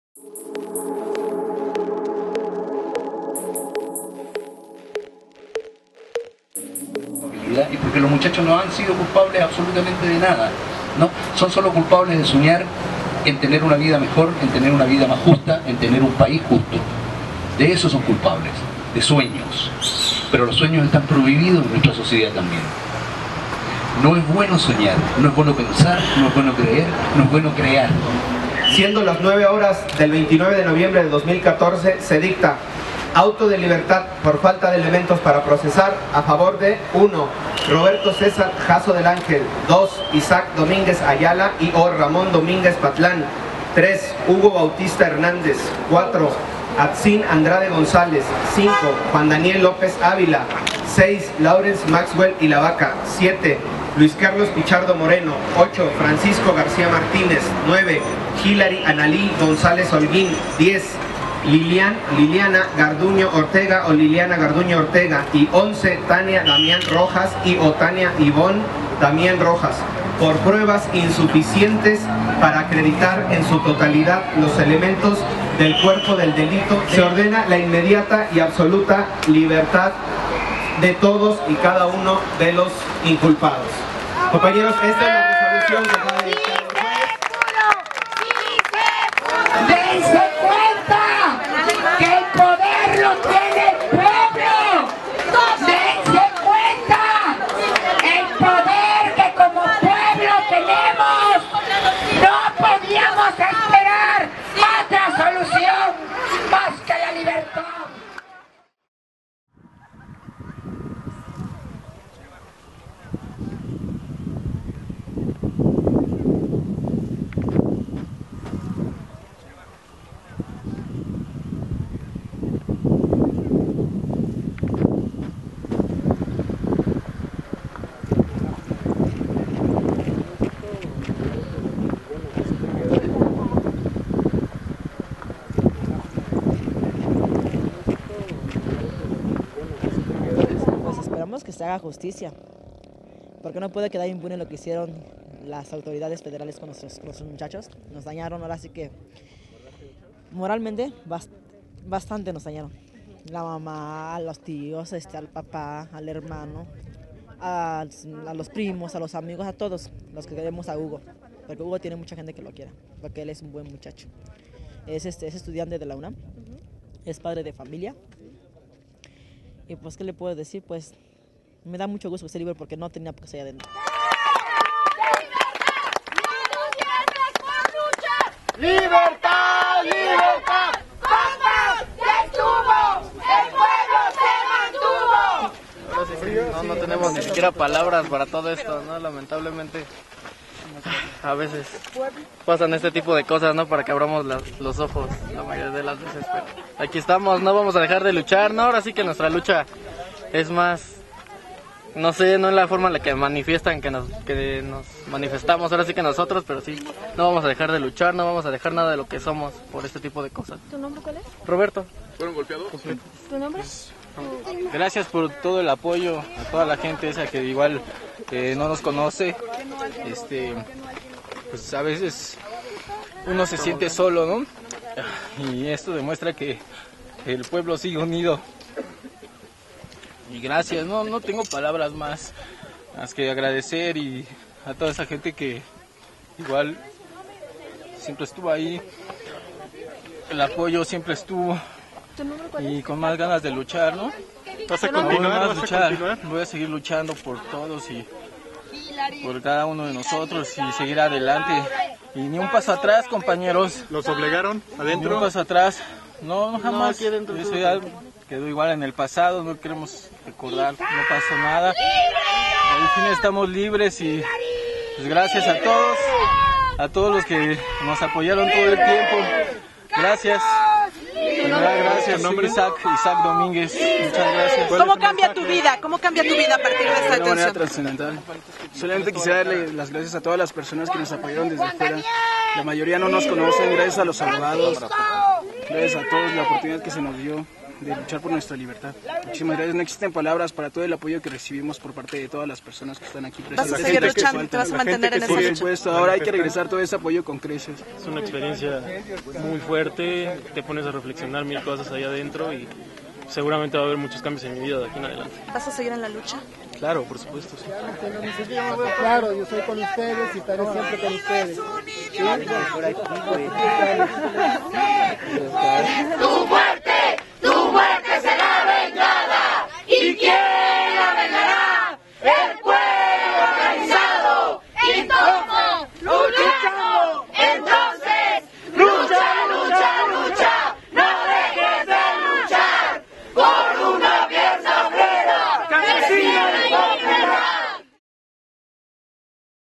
Salen del Penal de Villa Aldama detenidos en 20NovMx por falta de pruebas
Villa Aldama, Ver. 29 de noviembre 2014.- Tal y como se esperaba, la tarde de este sábado fueron puestos en libertad los 11 detenidos por el 20NovMX, acusados de tentativa de homicidio, motín y asociación delictuosa durante la marcha del 20 de noviembre en apoyo a los 43 normalistas desaparecidos de Ayotzinapa.